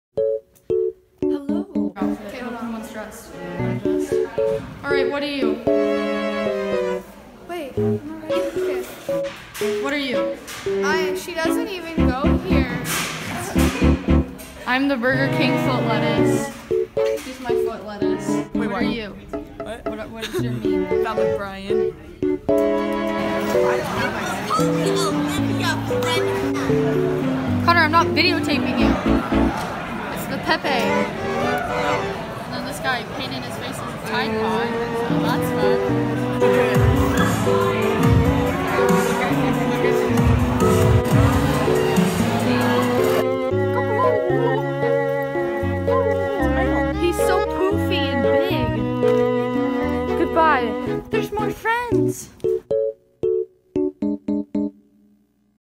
meme day but its mainly vines and three people dressed up
like three people dressed up I’m mad and I didn’t get footage of the other Burger King foot lettuce